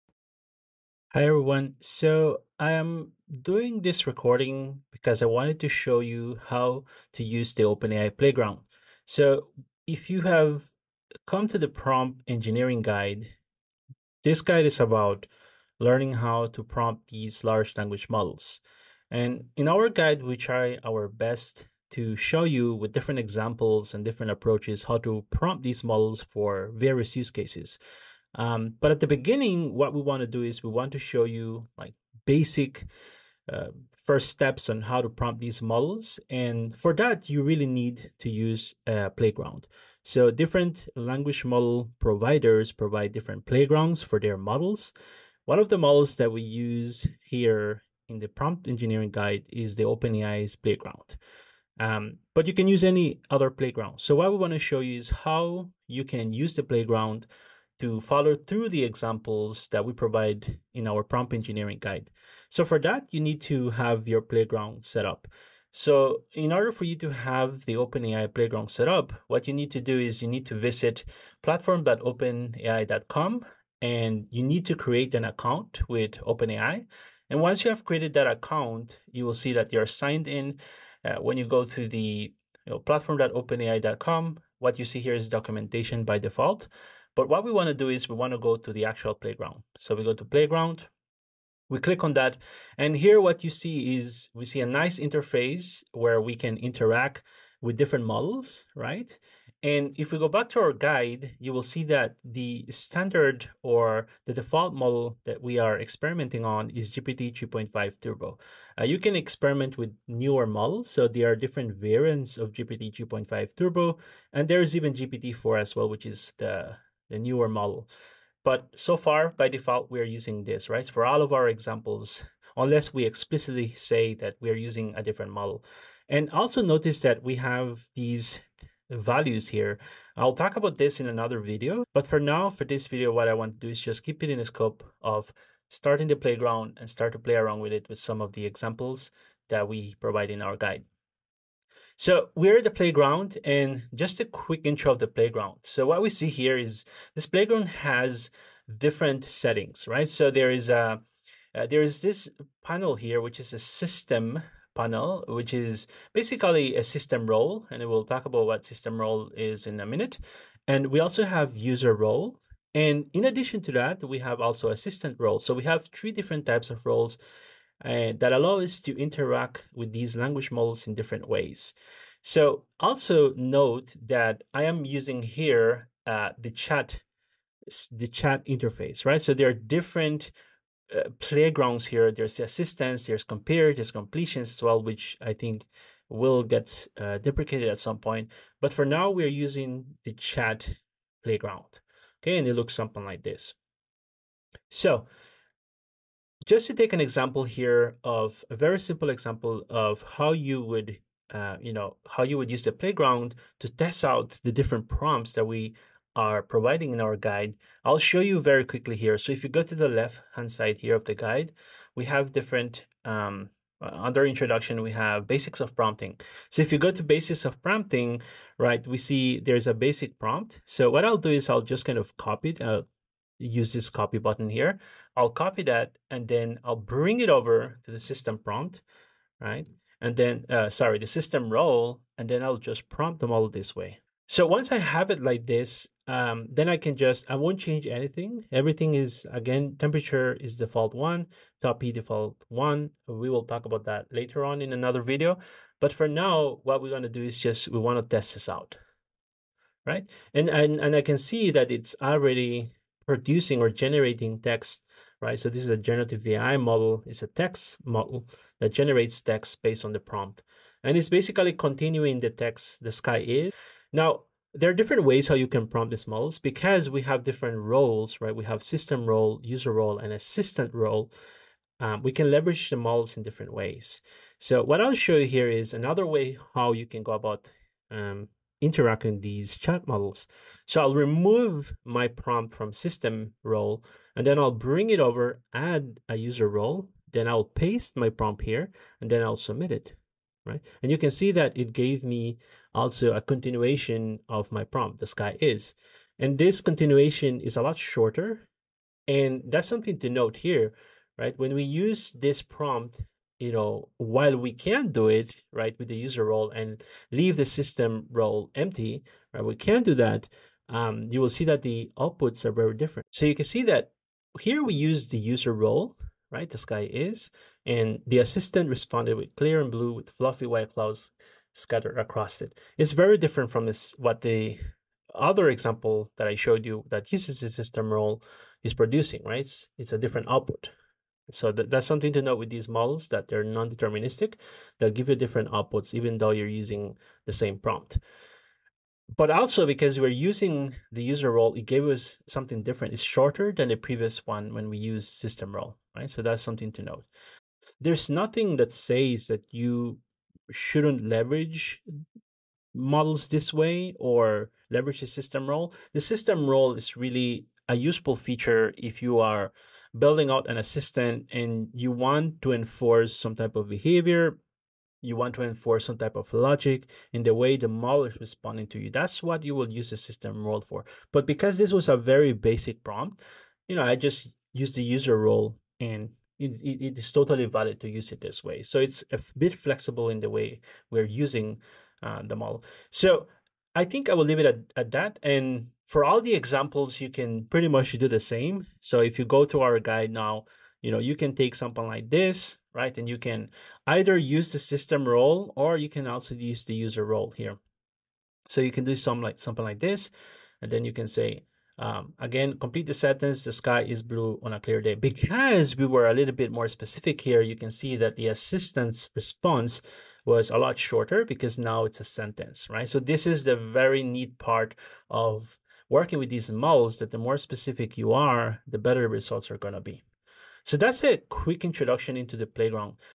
Getting Started with the OpenAI Playground
IMPORTANT: The discount is limited to the first 500 students.

This short tutorial provides a bit of guidance on how to get started with the OpenAI Playground.